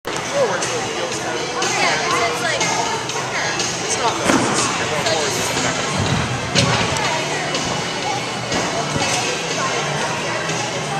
Kayla Arabian Double
She came to camp and asked to learn an Arabian double.  Check!  I used to coach her a few years ago. Glad to have the opportunity to coach her again. She listens well and picks up corrections quickly. She'll be a senior this year and trains at Olympia Gymnastics in Shelby Township.